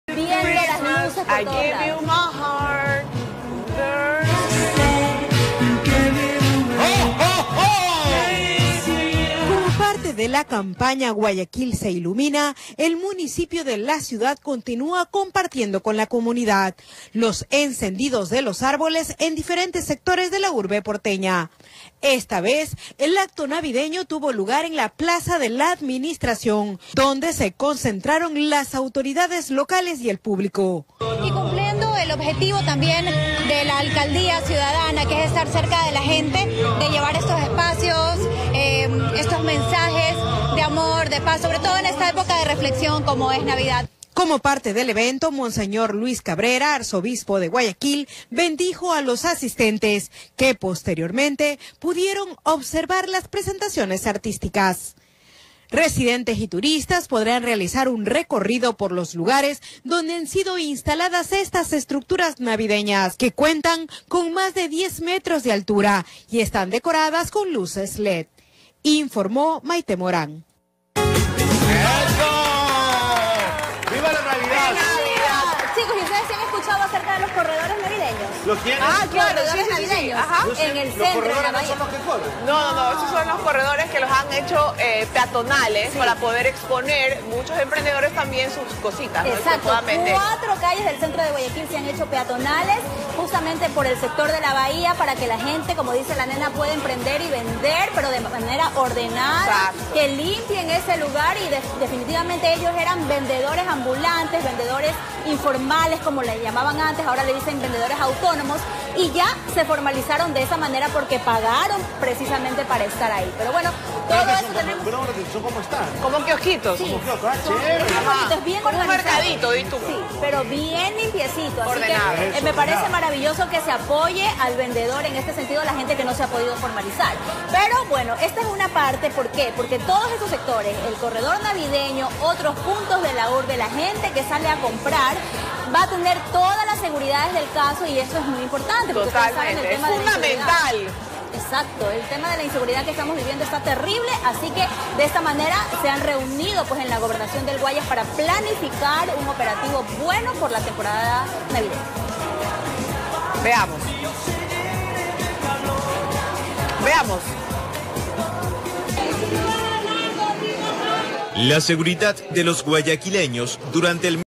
Se incendió el árbol en la Plaza de la Administración
Estructuras se mantendrán iluminadas durante las noches de este mes.

Con un conteo regresivo, en la plaza Rodolfo Baquerizo Nazur, situada en el Malecón del Salado, se dio el encendido de un árbol navideño, la noche de este jueves.